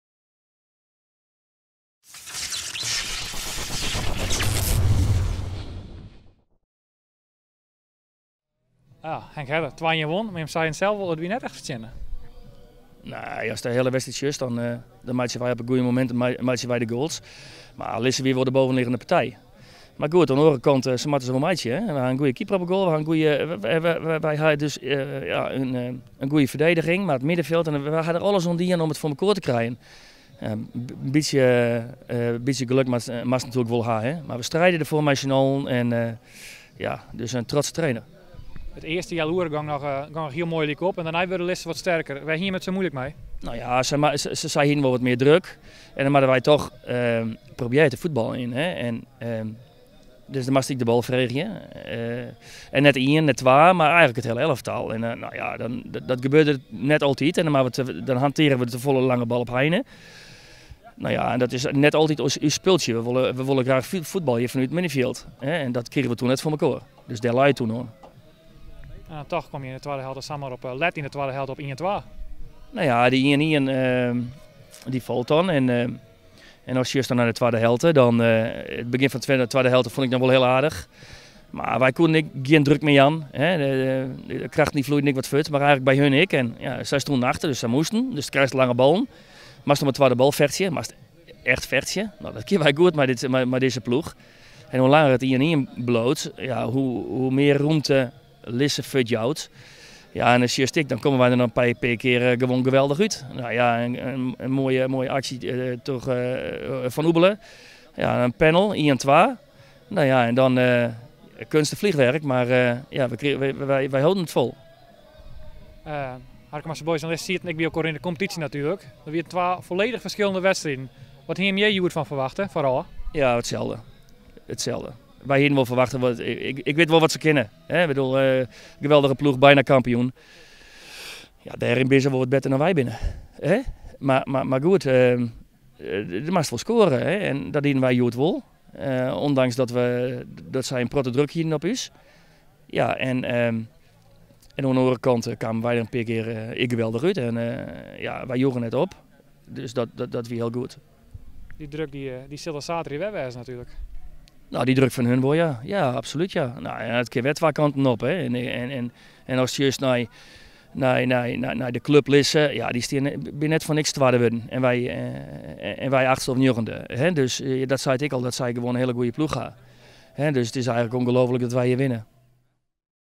Henk Herder: "Kunst- en vliegwerk, maar volgehouden"
Harkemase Boys zorgde voor een verrassing door in de nacompetitie met 1-2 te winnen op bezoek bij FC Lisse. Trainer Henk Herder was na afloop reëel.